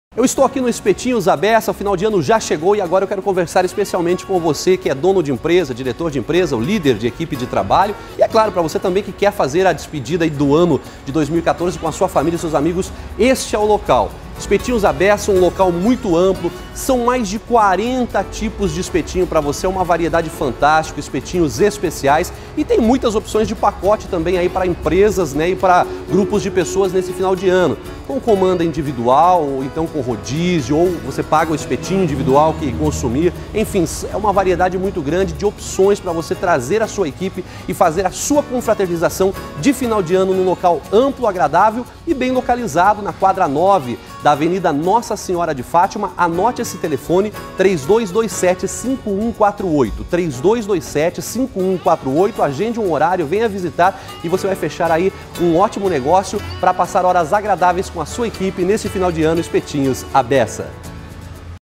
ALMOÇO EM BAURU - ESPETINHOS A BESSA - S 49
Almoço por kilo de segunda a sexta e espetinhos no jantar.

Endereço:
Avenida Nossa Senhora de Fátima, 9-55 Bauru - SP, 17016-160